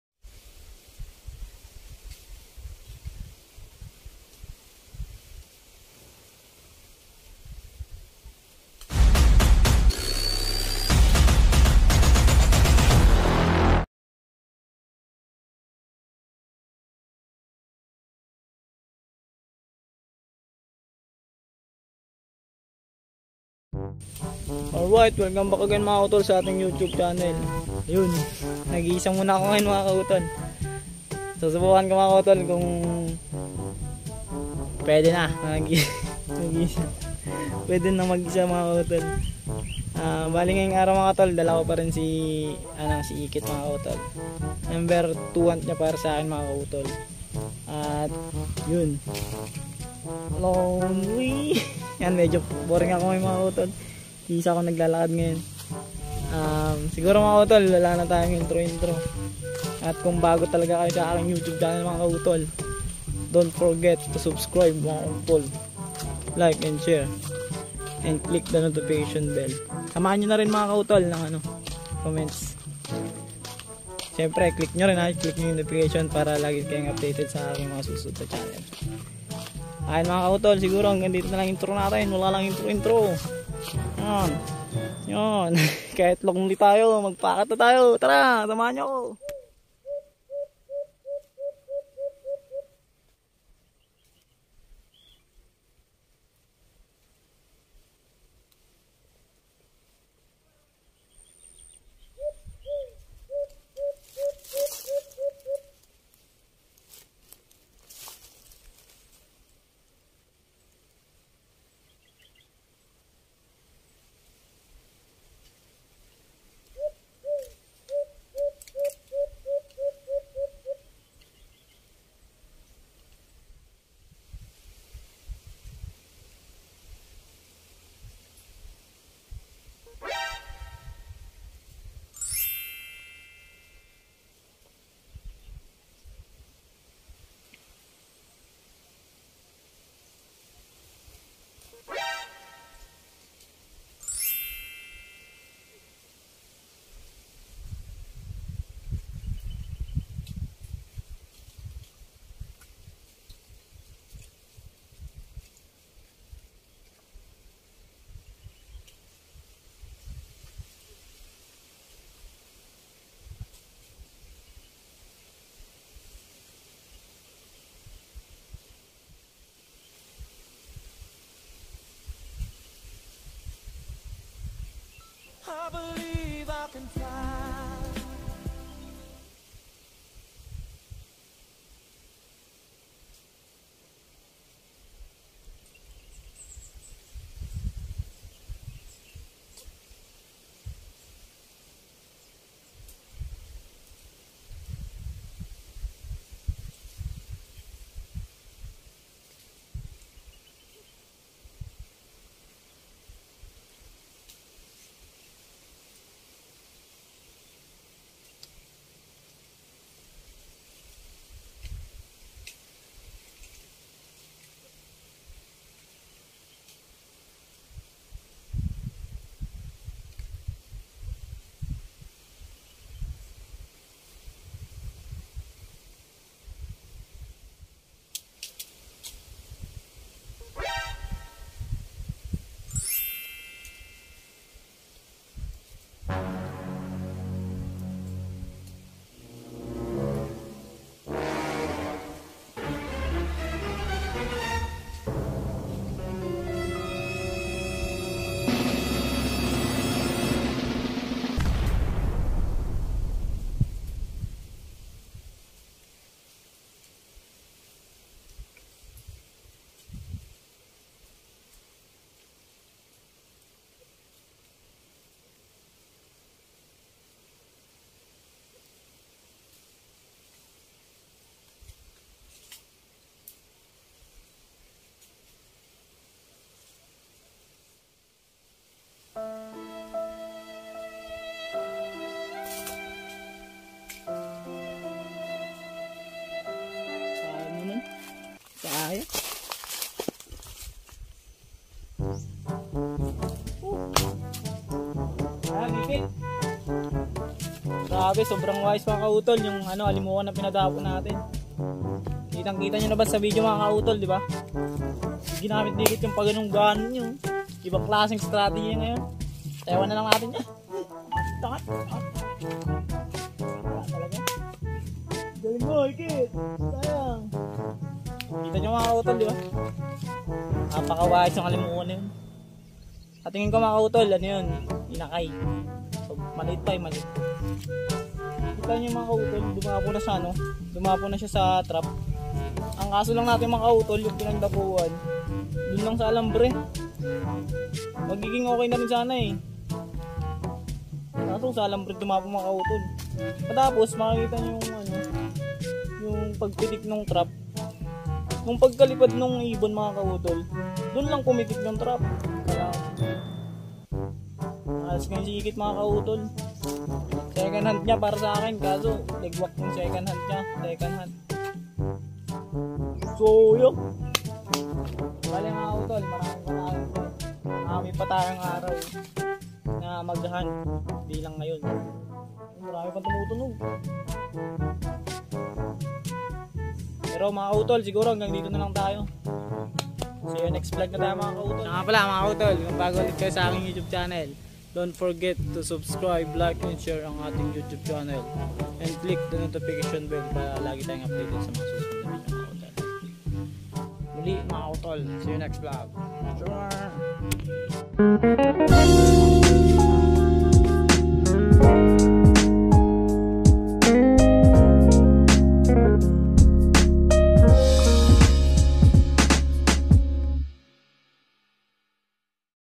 Hunt#3|Hunt#2forIKIT|Alimokon Hunting |Malupitang Huni ni IKIT |Lapiting Huni |Kautol |Solo Hunting
Solo Hunting muna tayo mga Kautol , Malupit na Huni ni IKIT . Sobrang Lapitin na Huni ni Ikit ..

Thanks.